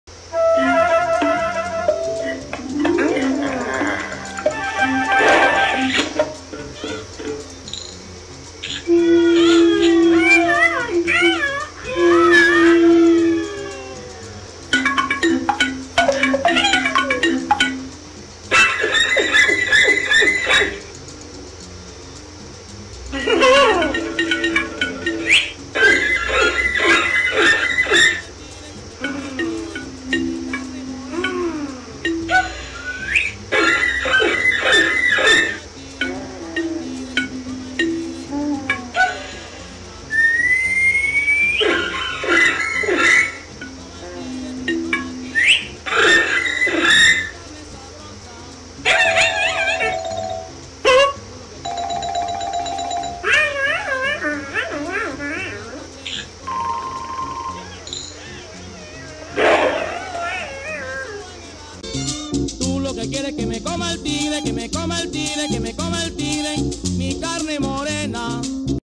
Grok !!
Un tigre de la era prehistórica se lleva al hombre, la mujer le suplica que no se lo coma porque perdería ..